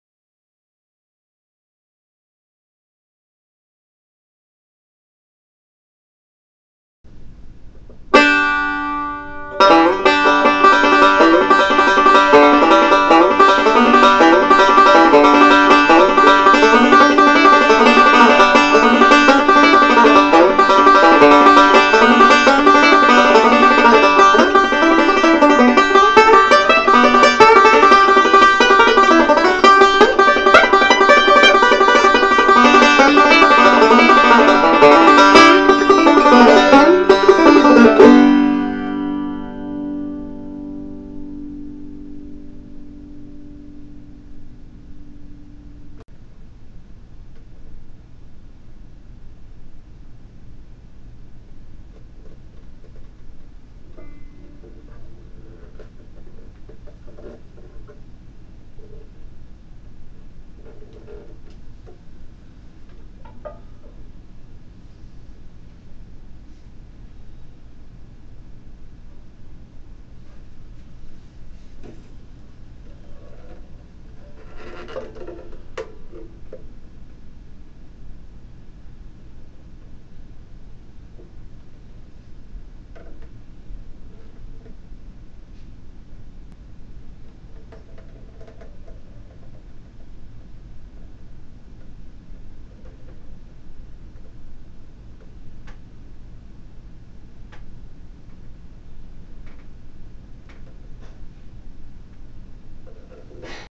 My 1965 Gibson RB500 - Sound Test and Close-Ups
I recently discovered this banjo while running wire for a light fixture in a customer's attic.  Didn't know what it was, but the members of Banjohangout helped me identify it.  Thanks!